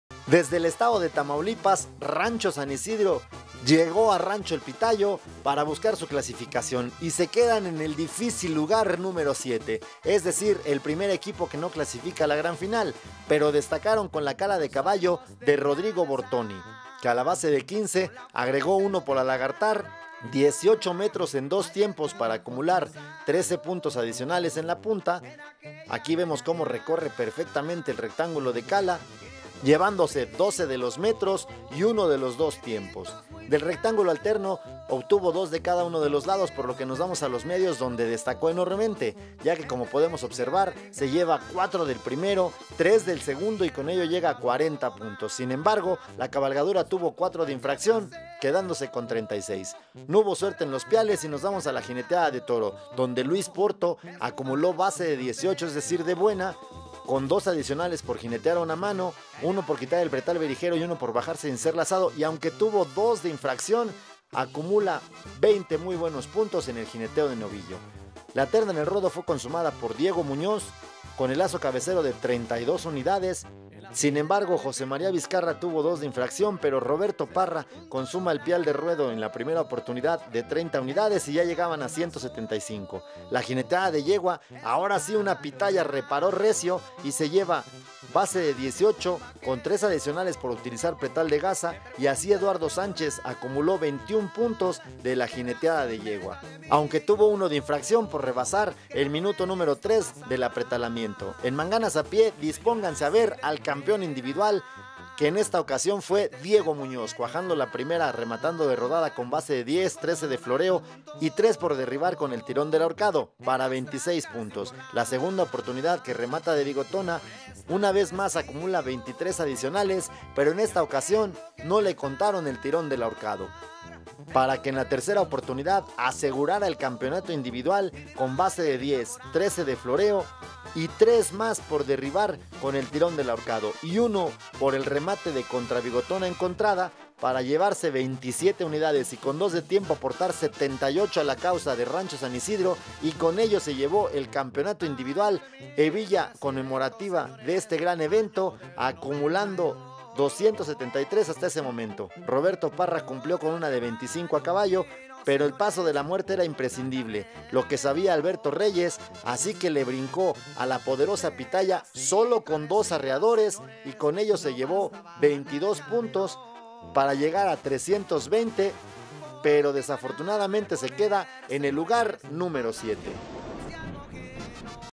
DLM288 180420 Bloque 2 - 6 VI Torneo del Millón Equipo Rancho San Isidro
VI Torneo del Millón "Rancho El Pitayo 2020" participando el equipo Rancho San Isidro, Tams.